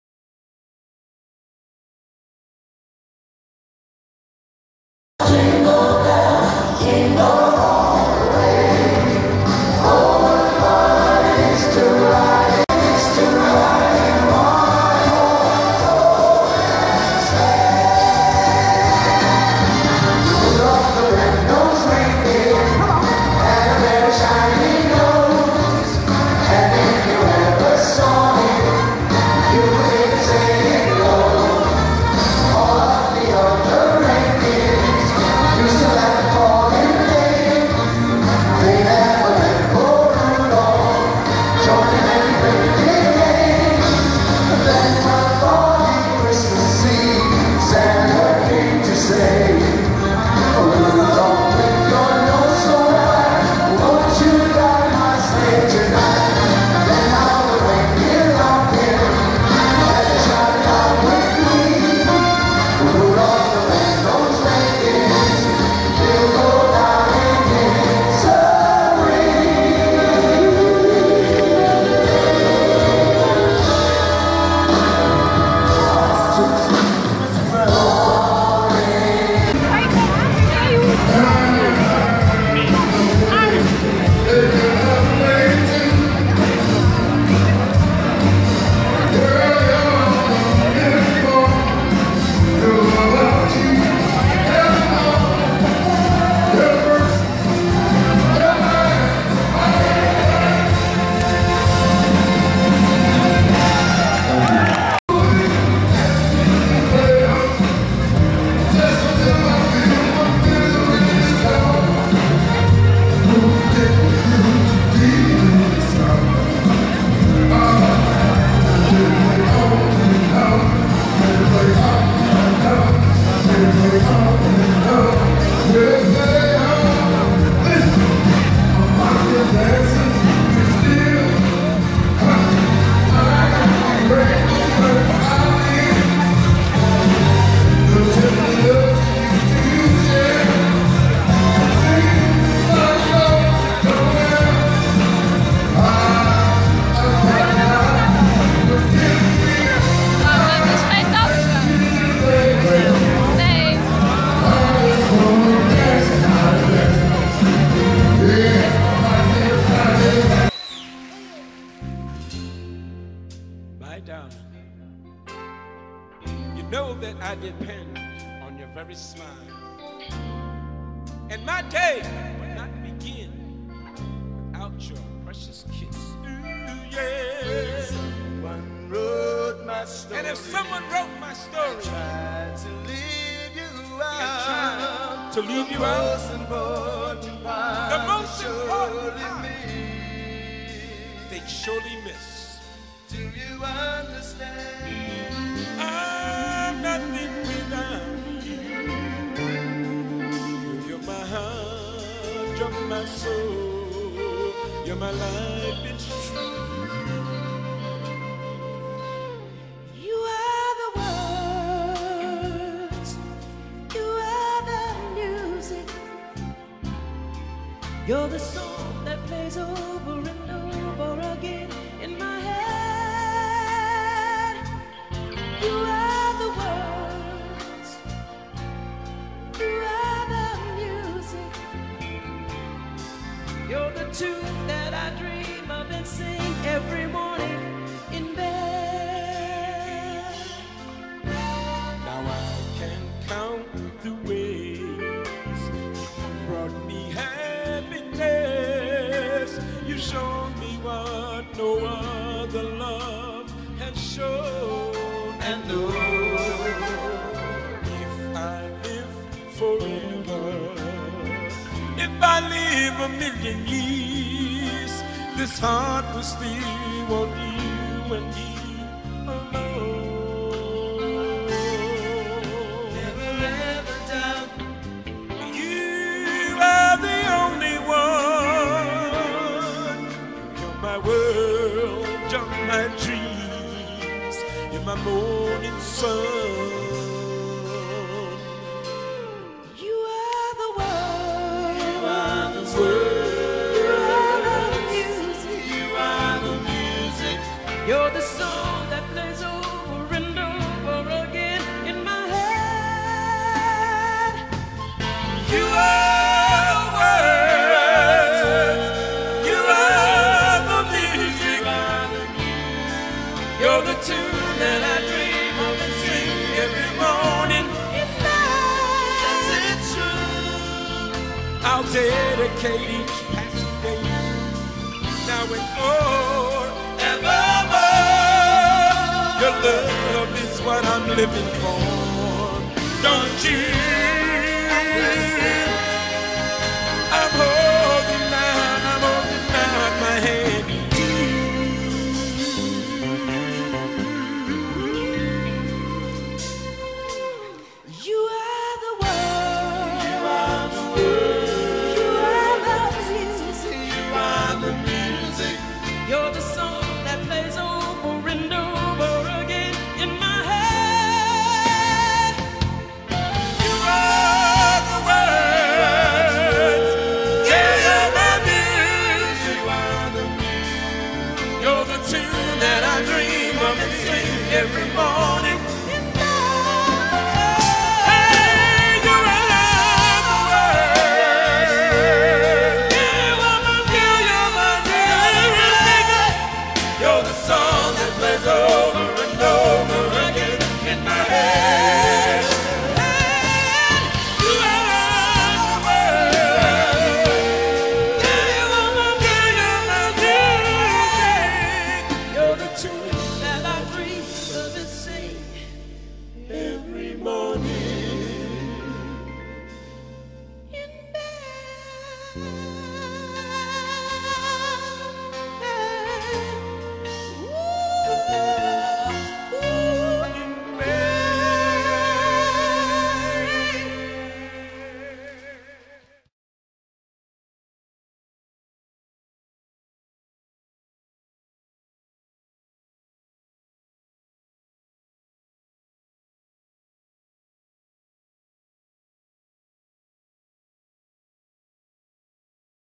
Max Proms 2015
Voorproefje van de MaxProms. De audio van het laatste stuk was zeer slecht en heb ik moeten vervangen.

Video maken en een laptop met Vista erop gaat niet meer zo goed samen. Met het programma VideoEditor is het nog wel te doen, maar dan krijg je zo''n groot watermerk erop in de gratis versie.